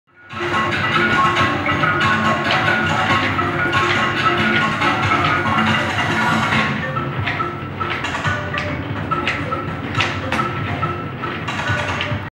OdySea Mirror Maze
Scottsdale, Ariz., is home to the OdySea Mirror Maze, a rainforest-themed mirror-filled labyrinth.